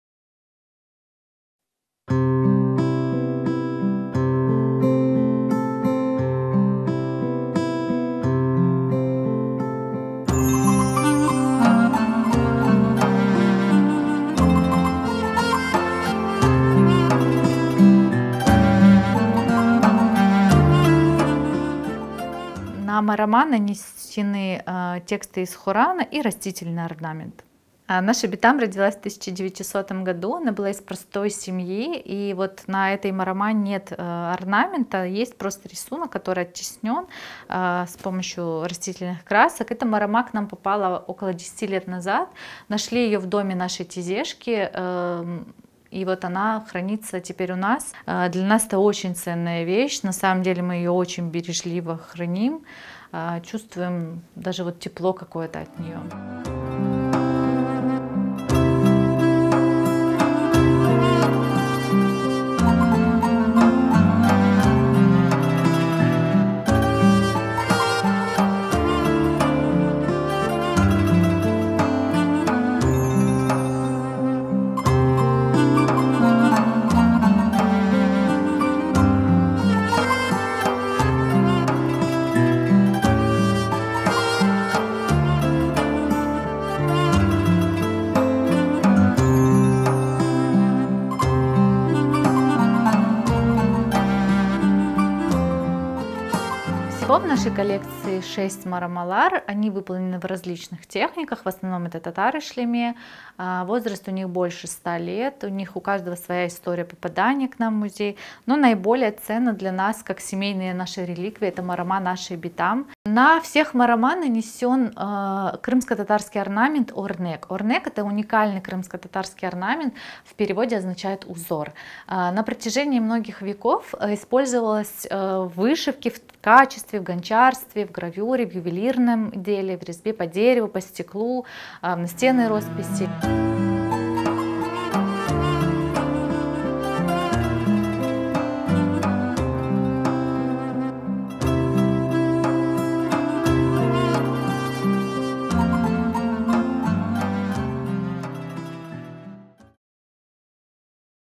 Марама со 130-летней историей
Известный коллекционер Айдер Халилов продолжает удивлять уникальными экспонатами. В его музее можно увидеть ценные исторические вещи, которые были приобретены в разных уголках Крыма. Ценной семейной реликвией и настоящей гордостью его коллекции считается головное покрывало – марама со 130-летней историей.